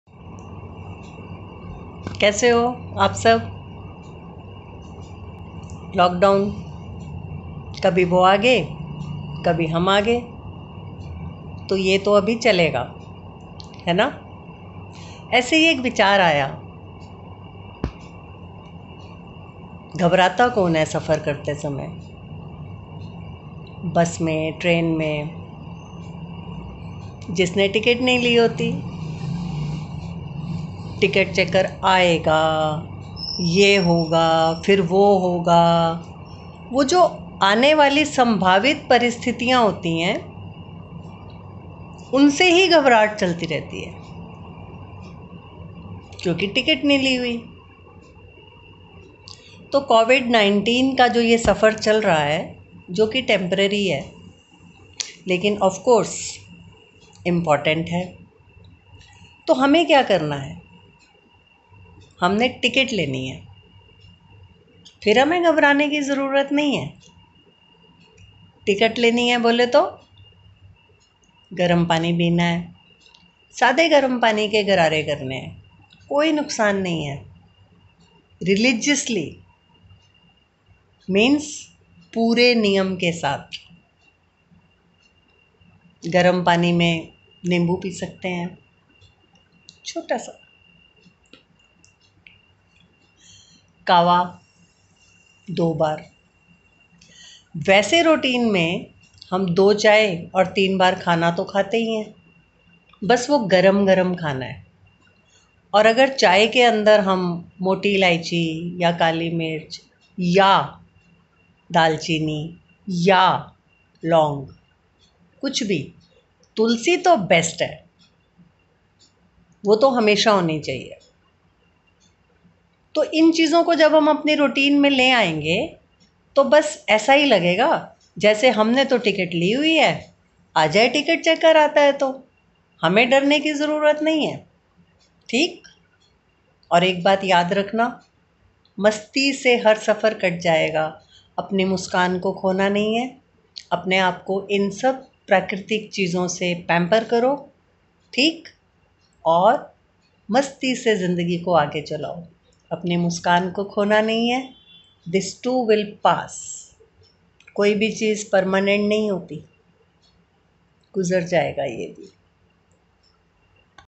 Ticket checker
She is senior doctor of Medanta Hospital treating corona patients successfully